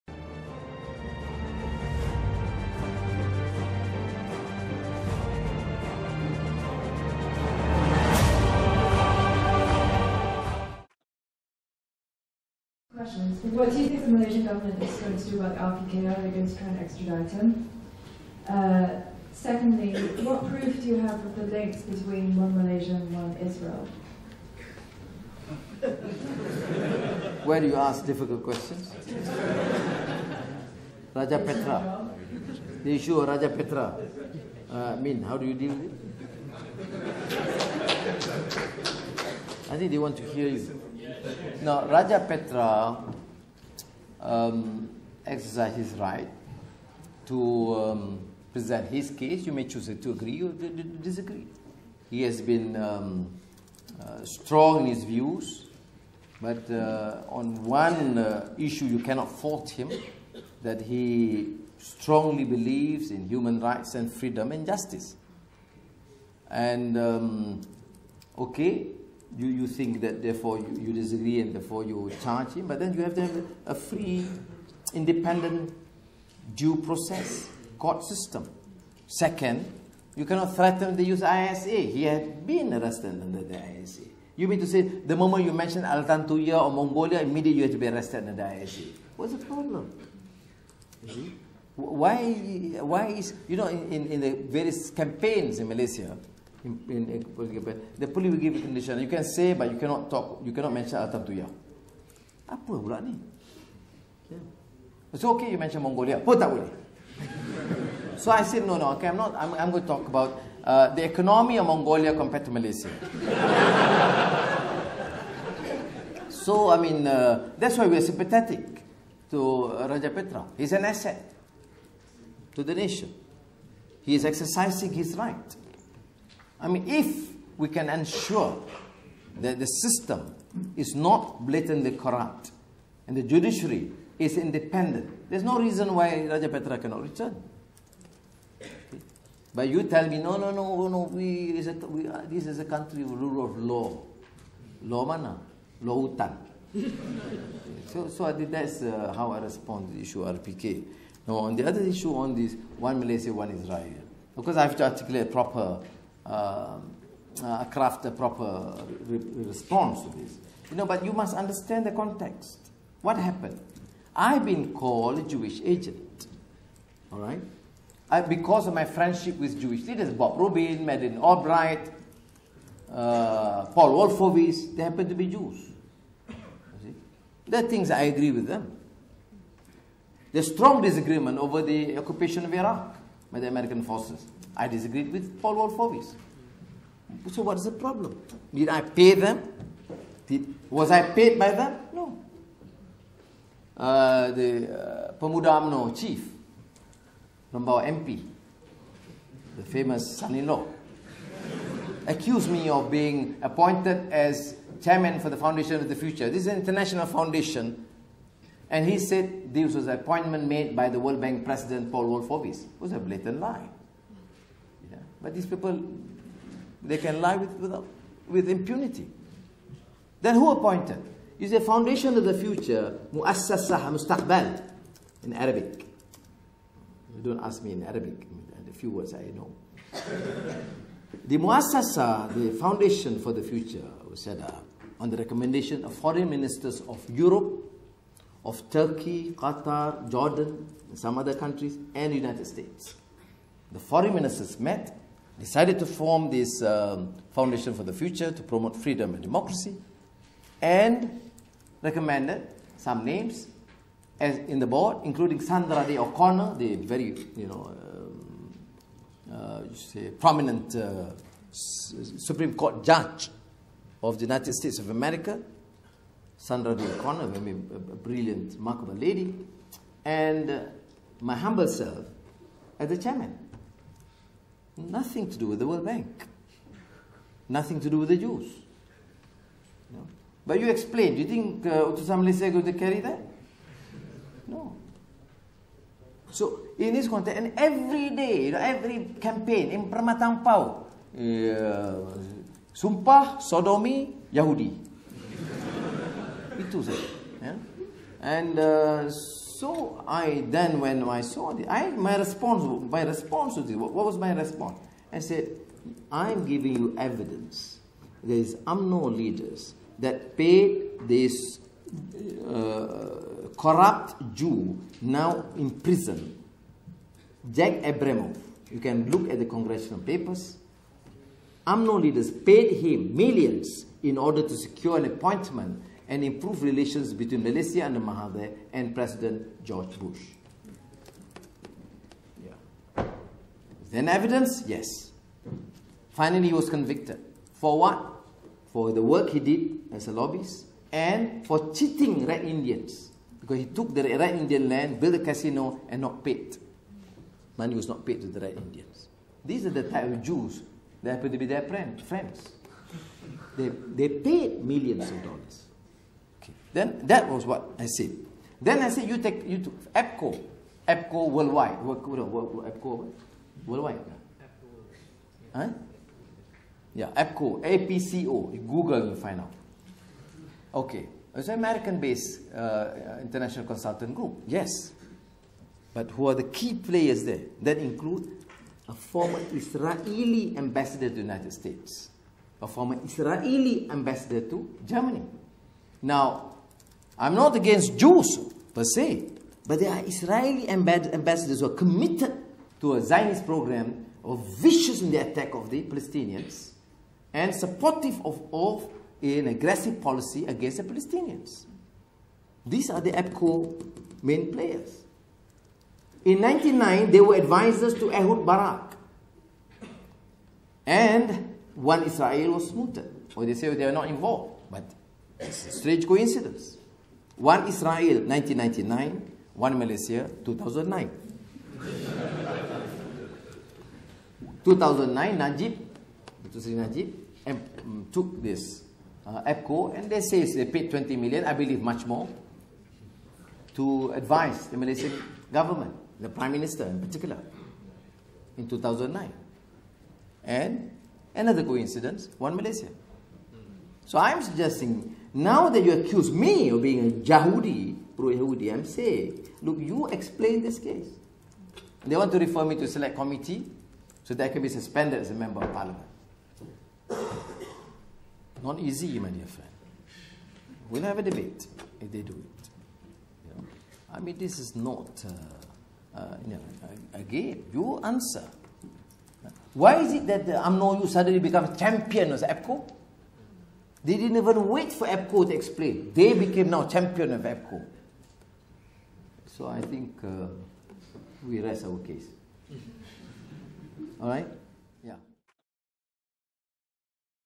Anwar Ibrahim: Raja Petra Is An Asset To The Nation
University of Westminster, London 19/03/2010  Q & A Session (Pt 6)